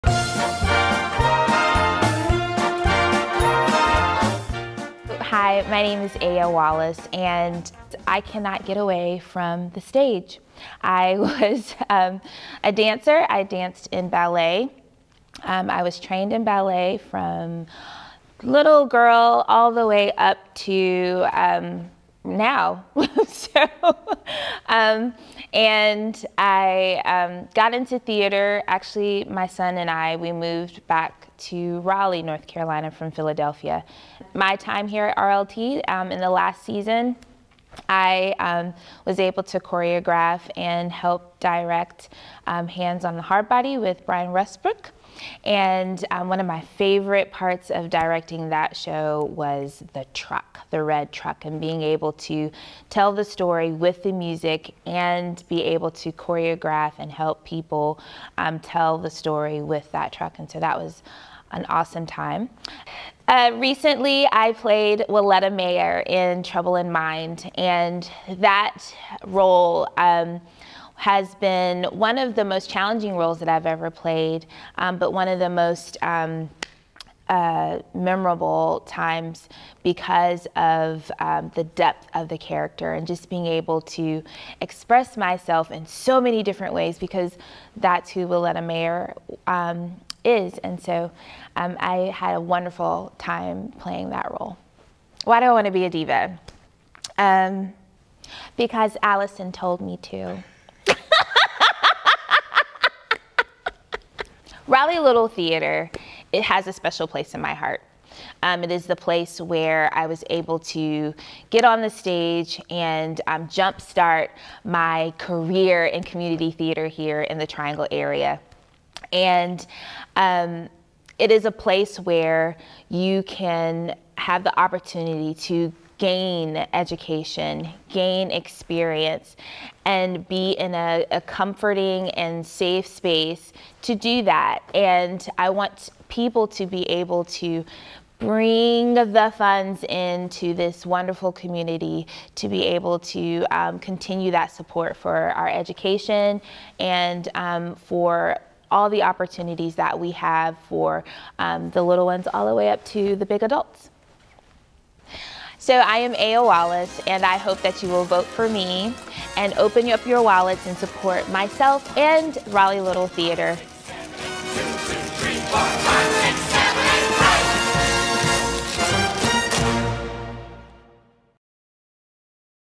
Aya Wallace for RLT Diva 2024!
Aya Wallace is a candidate for RLT's annual “Divas!” competition in 2024. “Divas! 2024” will take place in Raleigh Little Theatre’s Cantey V. Sutton Theatre on Saturday, May 11. Learn more about the event and donate to Aya’s campaign here: RaleighLittleTheatre.org/Divas2024